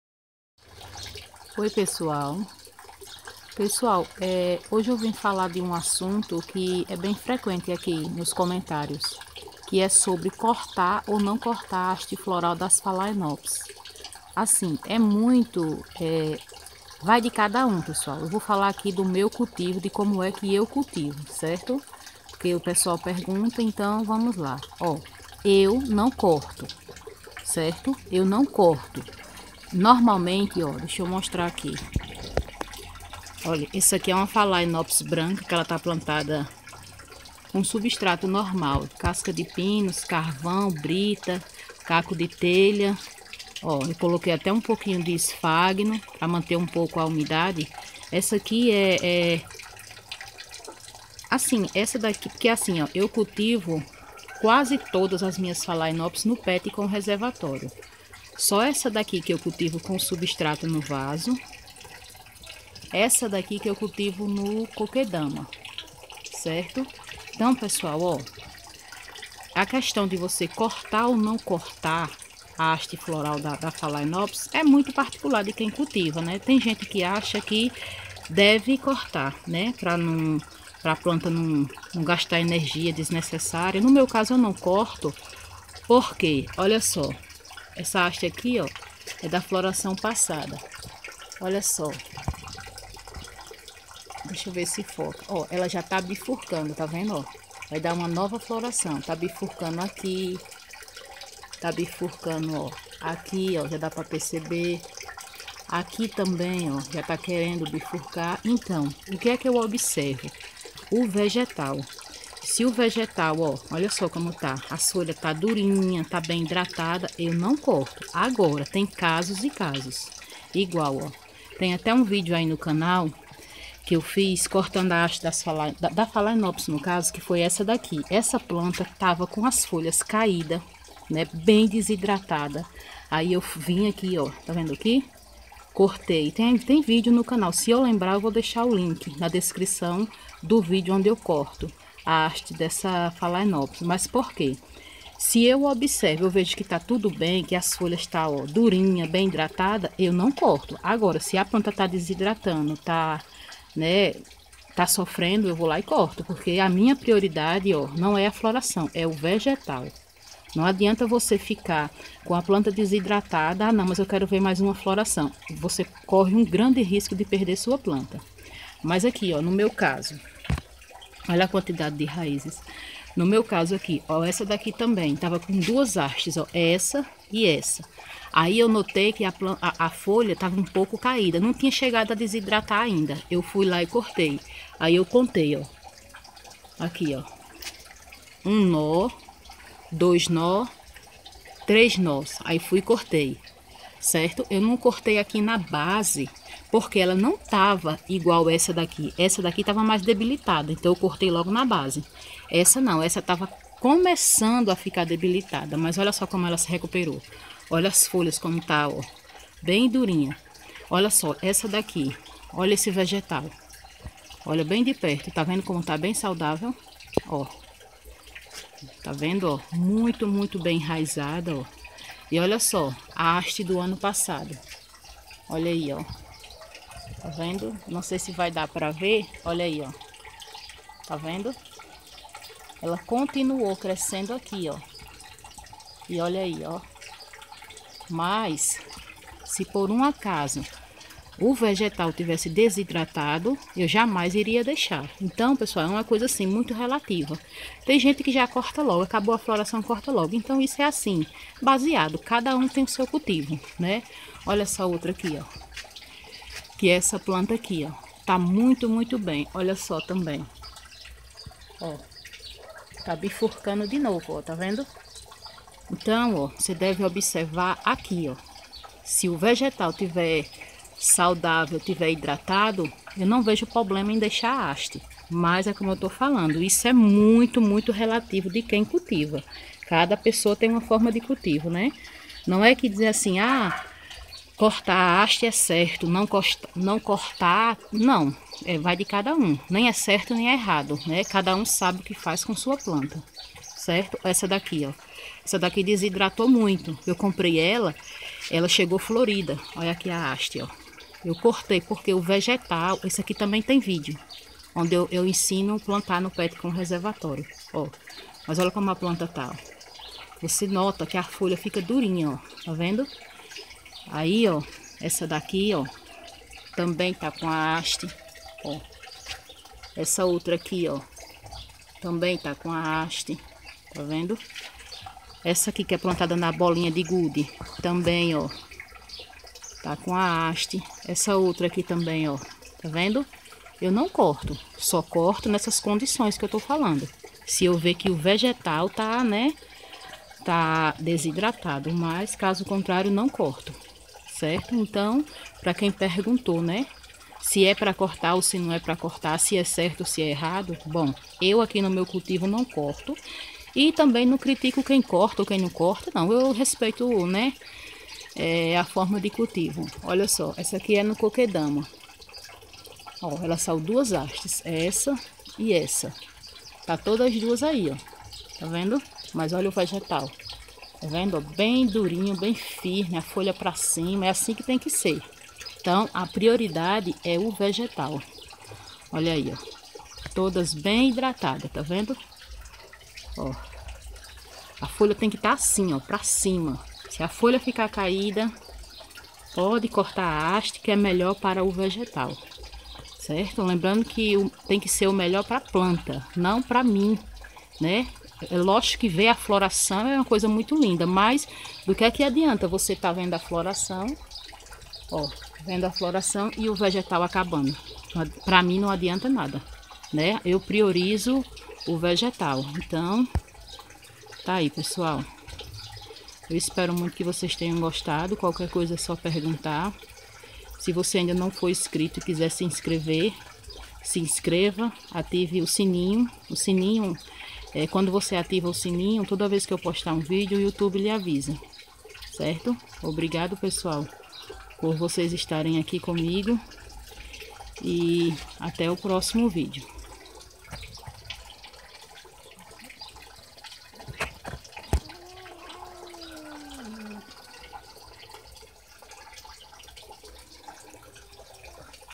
Orquídea Phalaenopsis
Oi pessoal bom dia?
Meu nome é Daiane, hoje vim falar vocês sobre cortar ou não cortar as hastes das phalaenopsis, espero muito que vocês gostem se gostou curta comente e compartilhe 😘